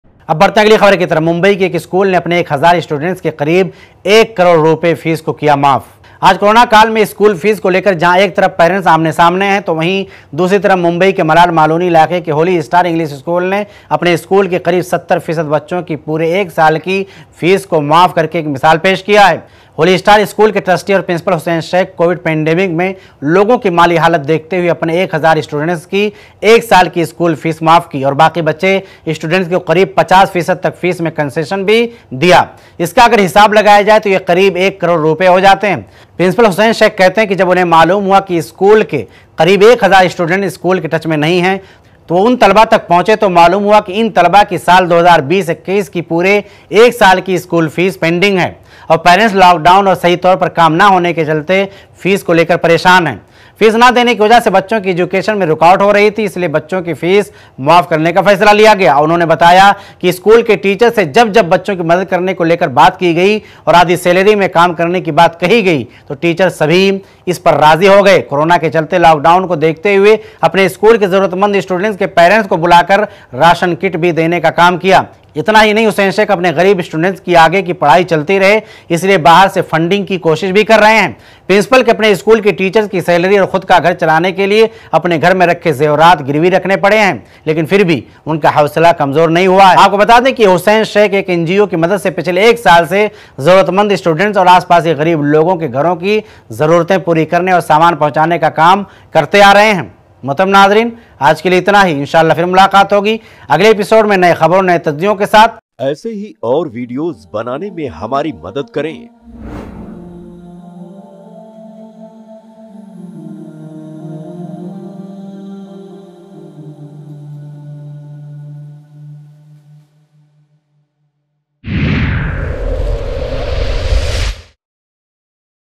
1 Carore Ki Fees Maaf Hussain Shaikh Ke Zariye Mumbai Ke Ek School Ki | Dr. Abdul Karim Alig
1 Carore Ki Fees Maaf Hussain Shaikh Ke Zariye Mumbai Ke Ek School Ki | Dr. Abdul Karim Alig | iPlus TV